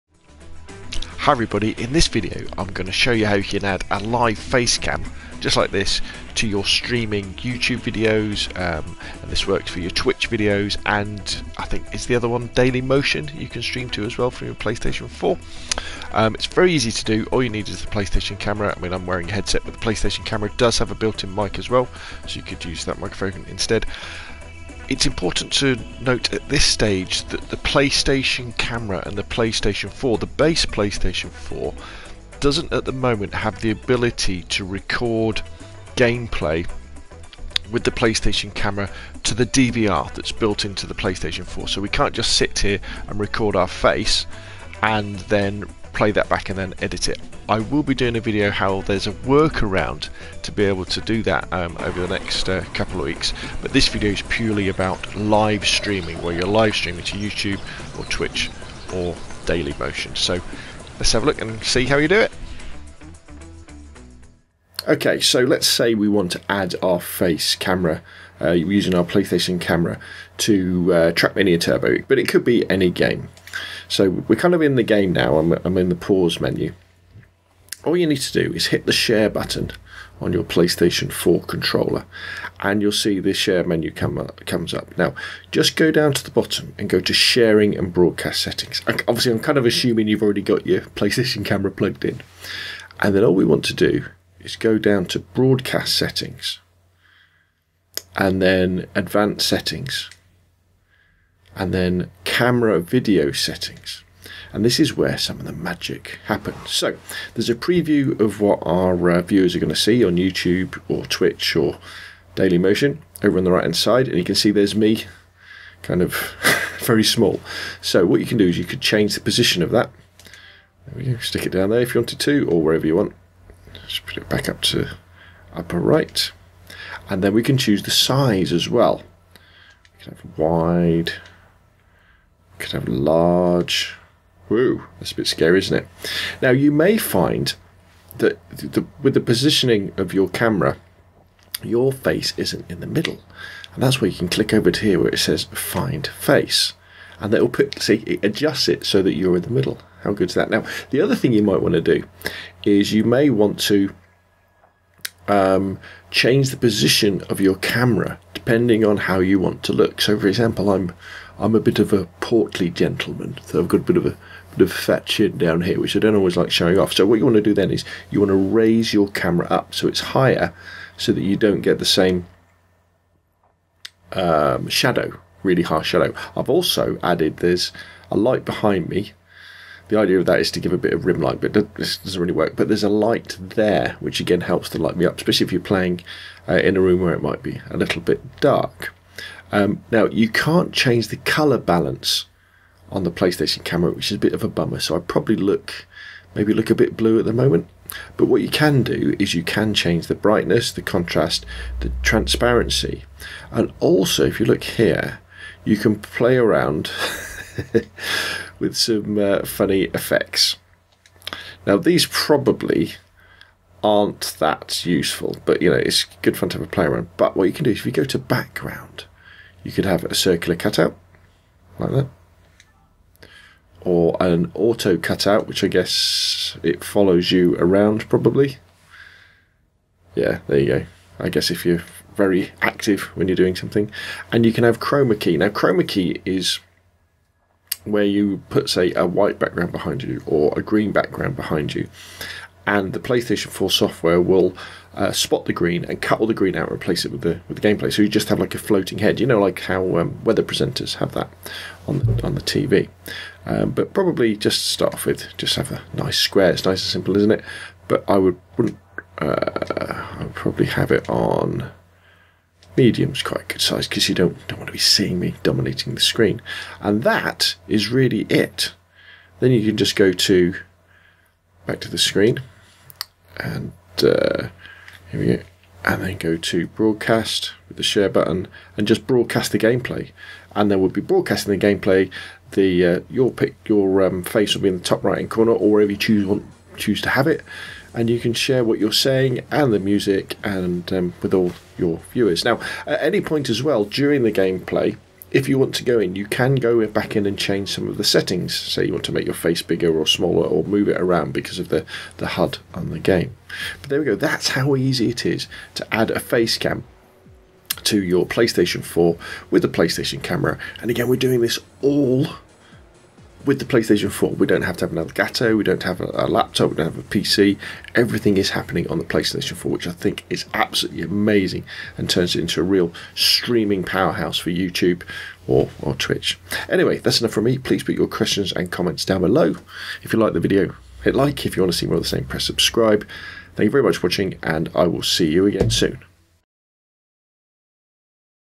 PS4 Playstation Camera: How To Add Face Cam Video To Your Live Youtube & Twitch Gameplay Streams
The PlayStation Camera can turn your PS4 into a Youtube & Twitch streaming powerhouse, adding PIP picture in picture face cam video to your live streams!
Thanks, Rob.